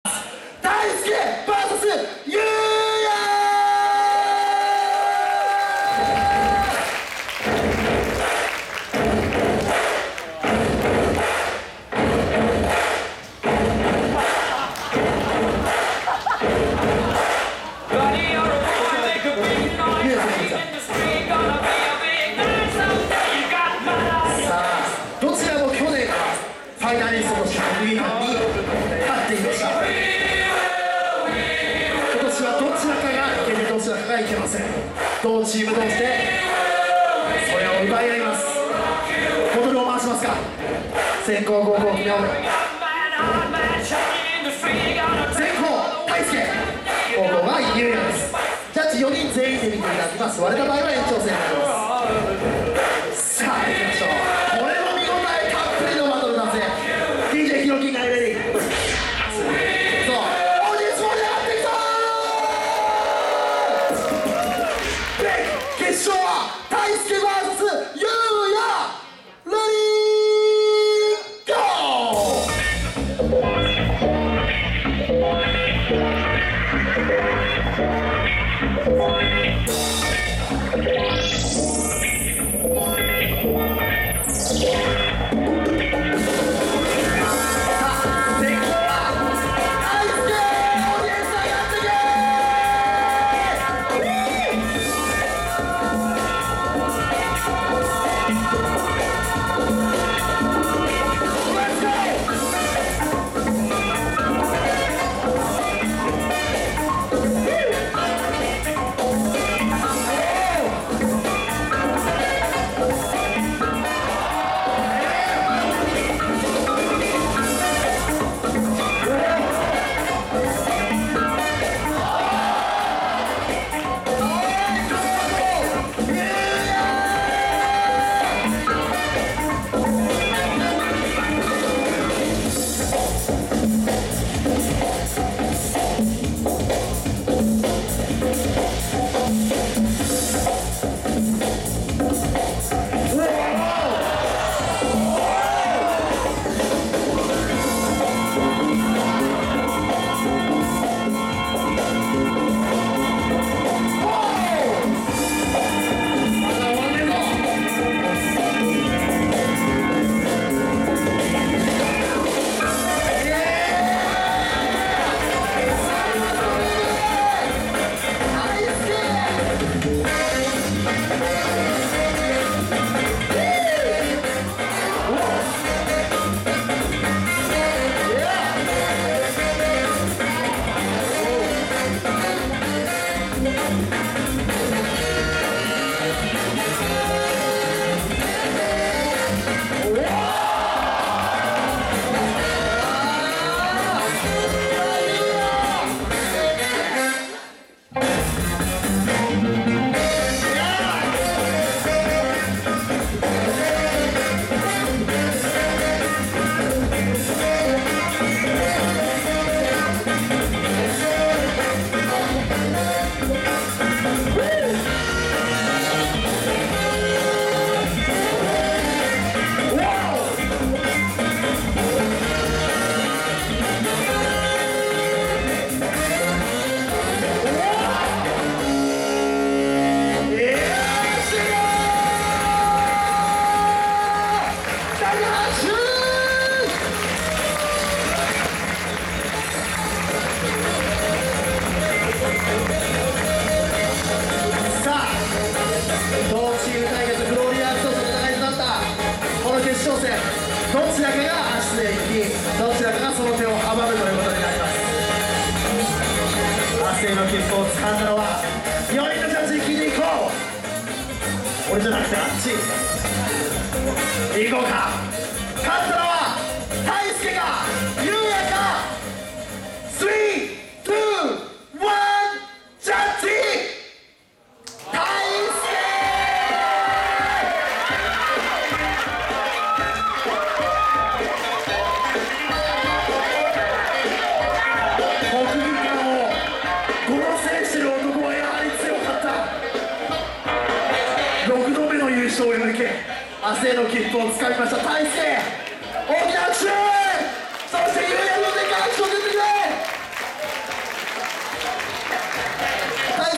2015.4.25 YU-YA VS TAISUKE BREAK FINAL [ 前日預選 ] DANCE @LIVE JAPAN FINAL 2015
前日預選DANCE LIVE JAPAN FINAL 2015 @両国国技館
【MC】IMAGINE